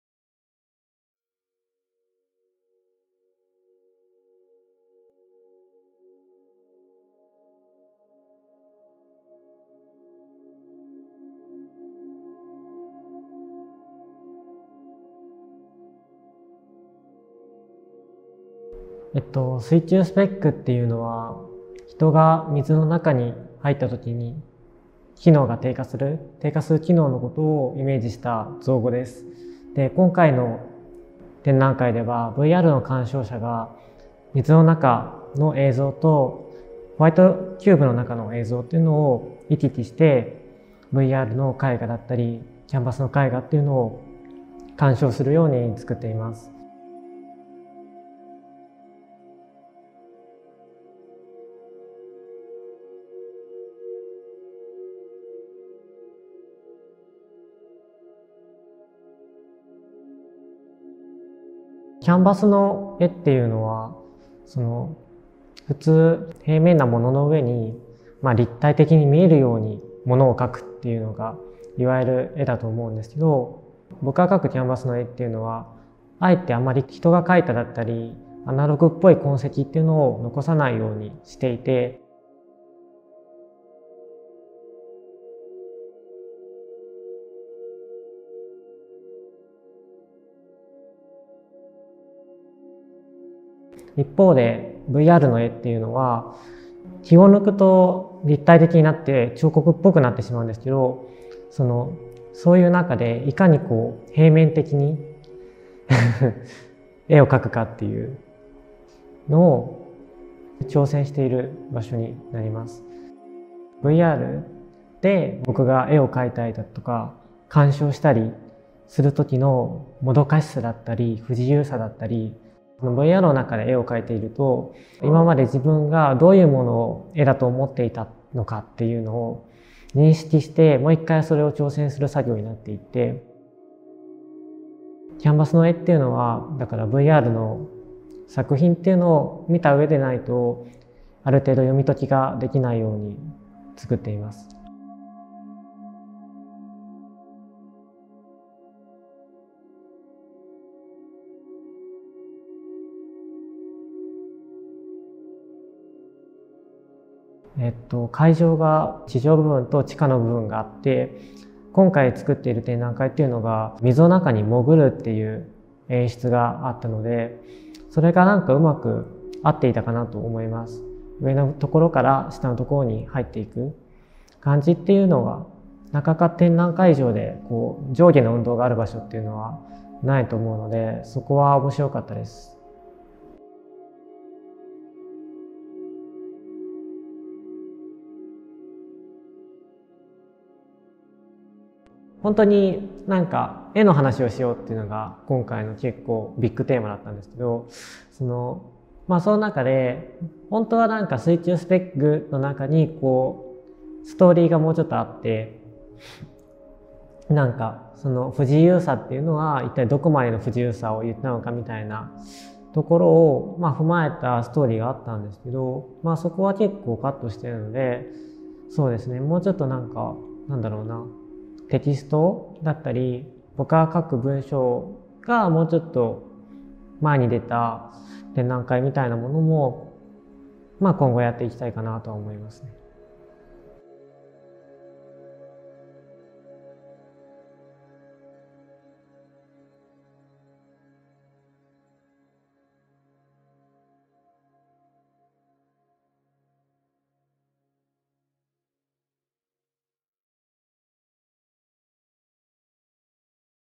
ザ・トライアングル 木村翔馬：水中スペック The Triangle Kimura Shoma：Virtually Unswimming
京都市京セラ美術館の「ザ・トライアングル」にて個展「水中スペック」（会期2020年9月19日〜11月29日）を開催したアーティストの木村翔馬が、本展のテーマである「水中スペック」、空間としてのザ・トライアングル、今後の活動について語りました。

収録日：2020年11月28日、12月14日
収録場所：京都市京セラ美術館

出演：木村翔馬
撮影・編集：片山達貴

制作：京都市
©︎2020 京都市京セラ美術館

Kimura Shoma’s solo exhibition Virtually Unswimming was on view between September 19 and November 29, 2020 at The Triangle in the Kyoto City KYOCERA Museum of Art. In this video, he talks about “Virtually Unswimming,” the main theme of this exhibition; The Triangle as an exhibition space; and future prospects as an artist.

Video recording conducted on November 28 and December 14, 2020, at The Triangle, Kyoto City KYOCERA Museum of Art.

Artist: Kimura Shoma
Video recording/editing: Katayama Tatsuki

Production: The City of Kyoto
©︎2020 Kyoto City KYOCERA Museum of Art